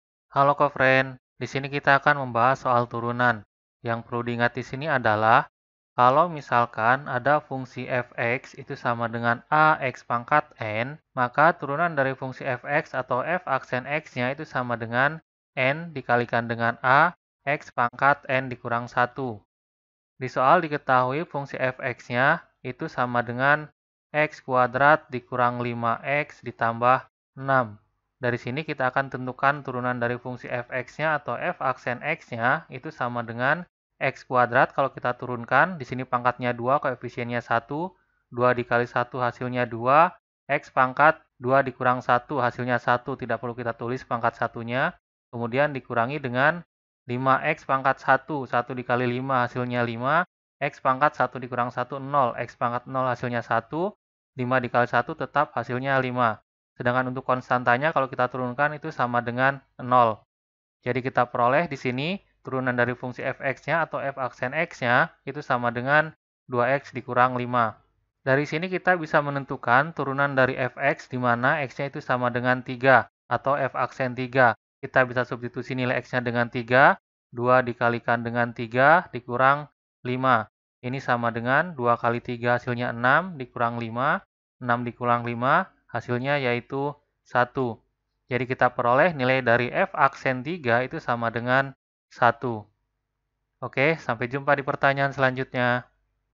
Diketahui f(x) = x^2 - 5x + 6 maka f'(3) =
Topik atau Materi: Turunan Fungsi Aljabar - Turunan - KALKULUS - Matematika Kelas 11 SMA -

Sumber Soal: Matematika Kemendikbud Rumah Belajar SMA , Kemendikbud

00:00 : Bedah Soal
01:00 : Konsep, rumus dan pengertian Turunan Fungsi Aljabar
01:18 : Langkah penyelesaian soal
00:36 : Jawaban akhir
01:06 : Penutup